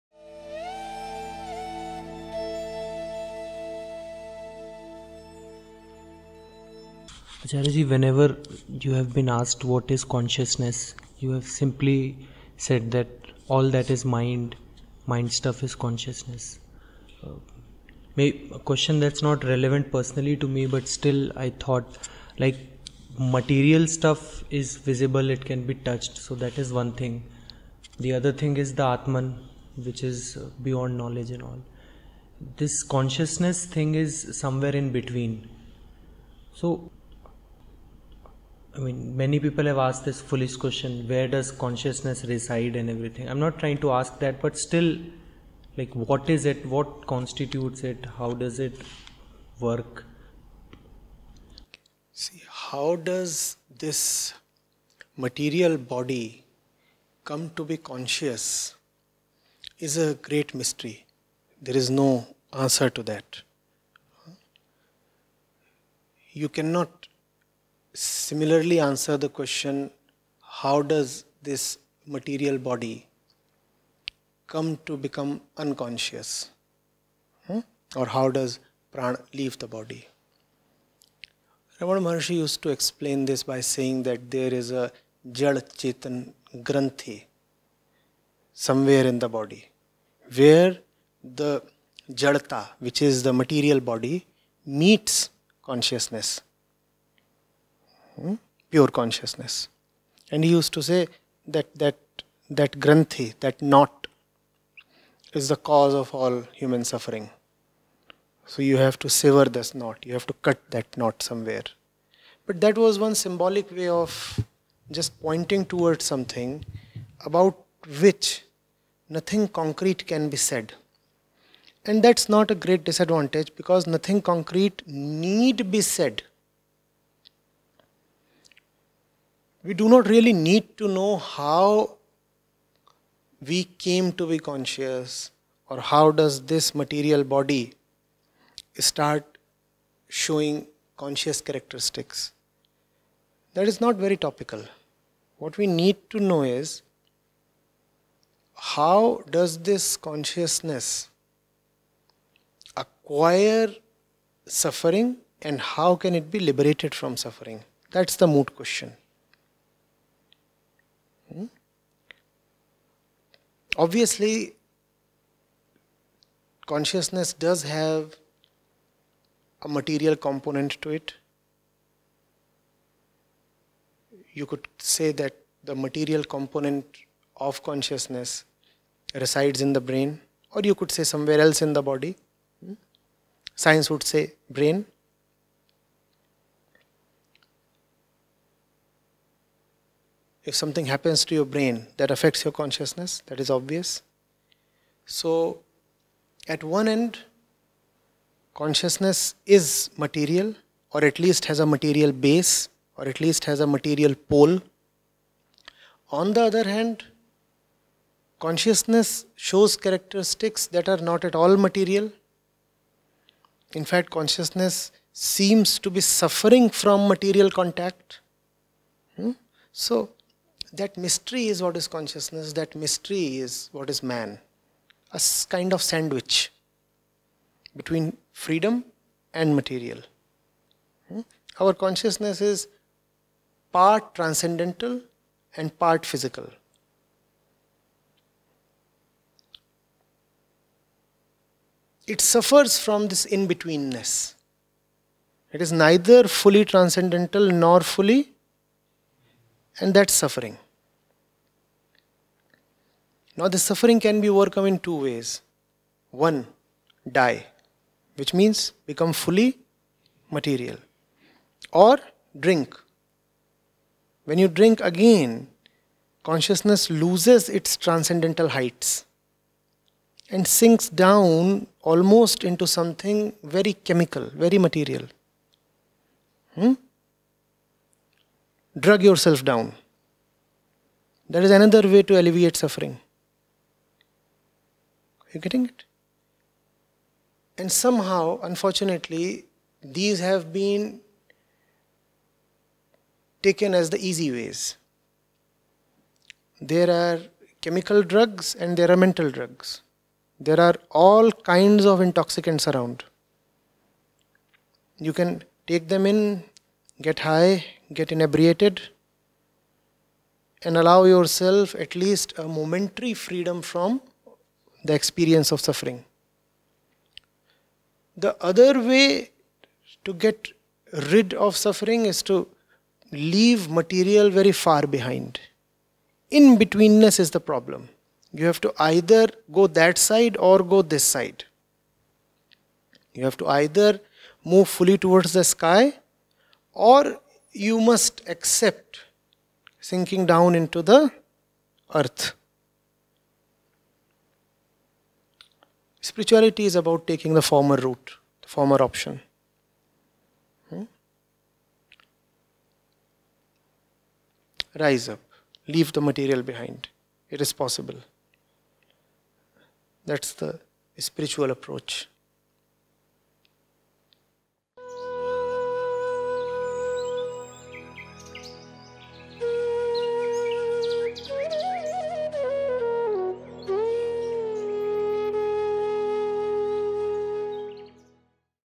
What is consciousness: physical or nonphysical? || Acharya Prashant (2020)
Video Information: Shastra Kaumudi Live, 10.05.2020, Greater Noida, Uttar Pradesh, India

Context:
~ What is consciousness?
~ Is consciousness physical or nonphysical?
~ What does it mean to go beyond ordinary consciousness?